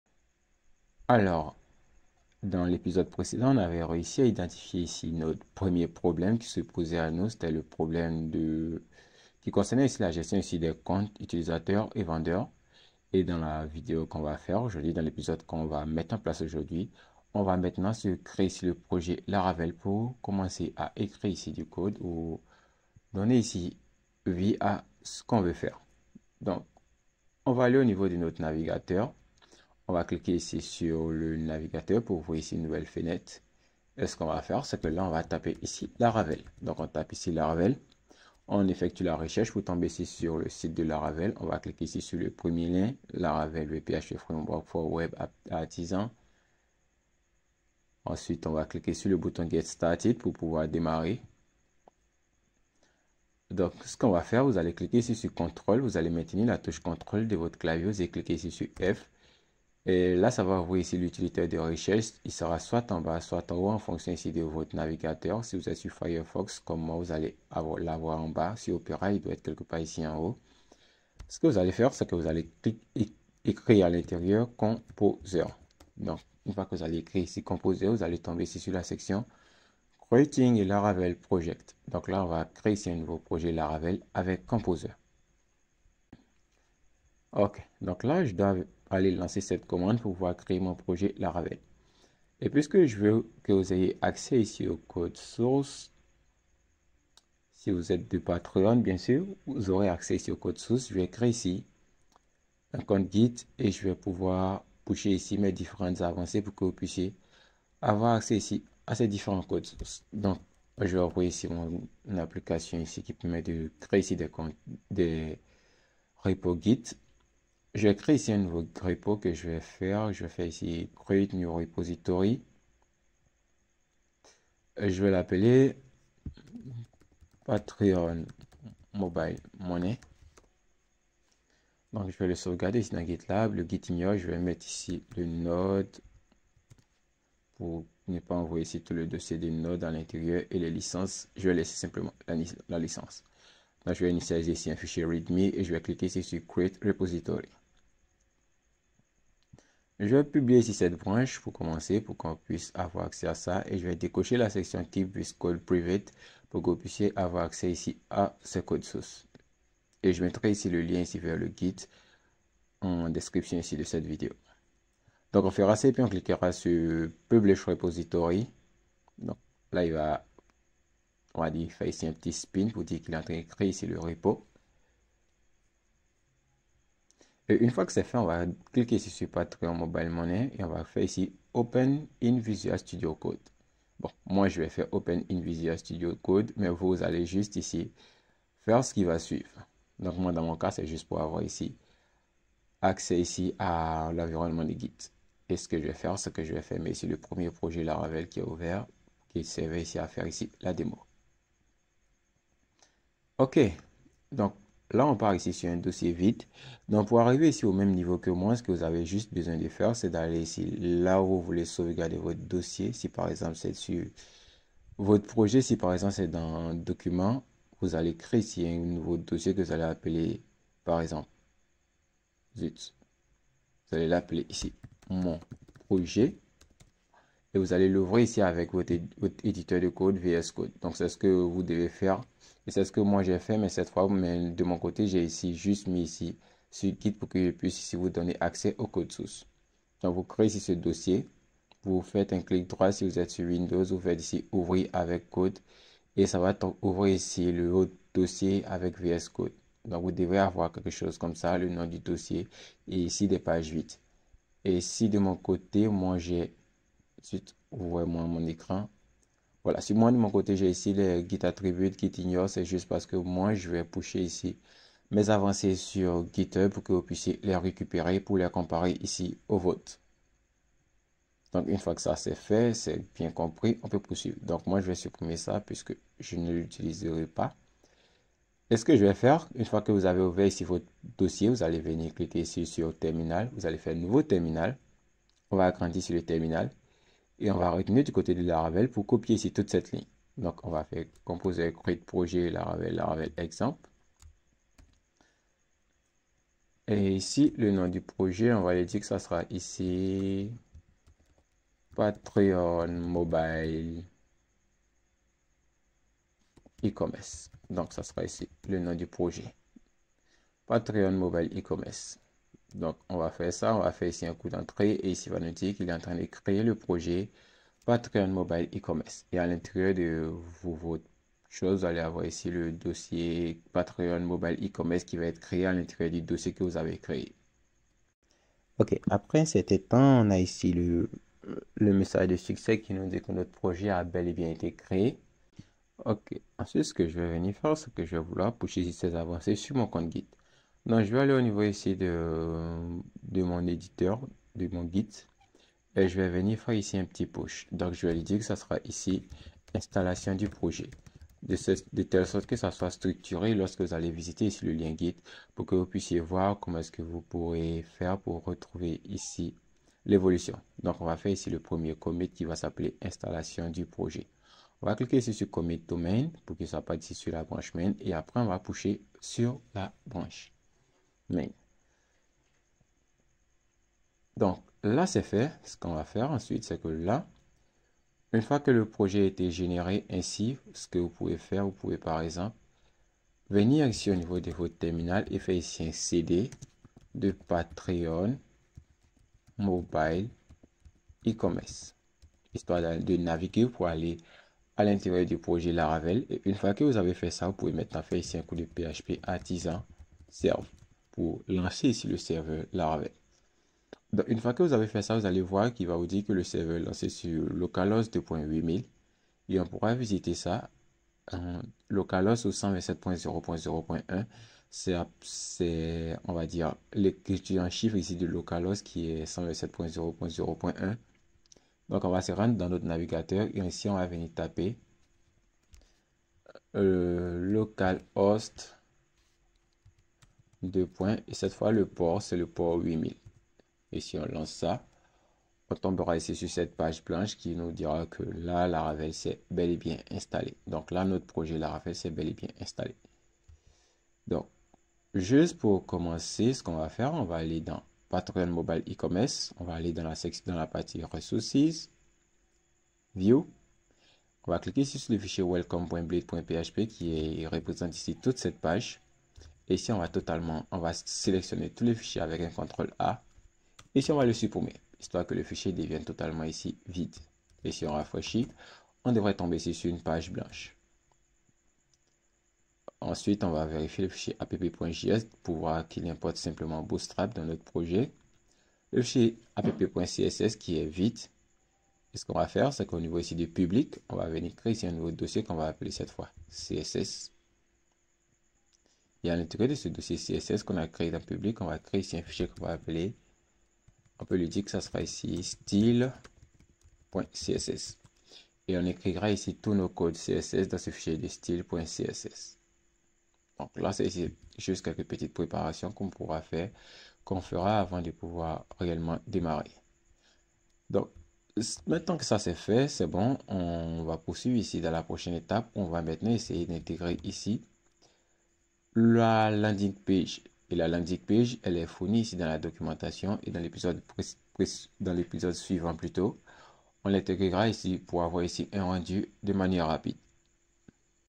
Laravel Multi Vendor - #2 - Creation du projet (Code source indisponible)
Bienvenue dans cette présentation dédiée à l'intégration d'un système de paiement mobile money dans votre application Laravel, spécifiquement conçue pour un site e-commerce de type multi-vendeur. À la fin de cette formation, vous serez en mesure de :

    🛡️ Comprendre le système d'authentification pour plusieurs types de comptes (utilisateurs, administrateurs, vendeurs).

    👤 Créer un système d'authentification adapté à différents profils d'utilisateurs.

    🛍️ Mettre en place un mini site e-commerce robuste.

    💸 Intégrer les paiements mobiles money dans une application Laravel.

    🌊 Intégrer les paiements Wave Money dans une application Laravel.

    💼 Gérer de manière efficace le processus de paiement.

    🔐 Appliquer les bonnes pratiques de développement sécurisé avec Laravel.

Cette série de formation vise à vous fournir les compétences nécessaires pour maîtriser chaque étape du processus, allant de la mise en place de l'authentification à la gestion sécurisée des transactions financières, tout en mettant l'accent sur le développement d'un site e-commerce performant. Nous sommes impatients de vous accompagner dans cette aventure passionnante vers la création d'applications Laravel sécurisées et fonctionnelles. 🚀

📙Code source du projet :
🗃️Ressources de la vidéo :
🔗Lien vers la communauté :
🔗Lien vers Discord :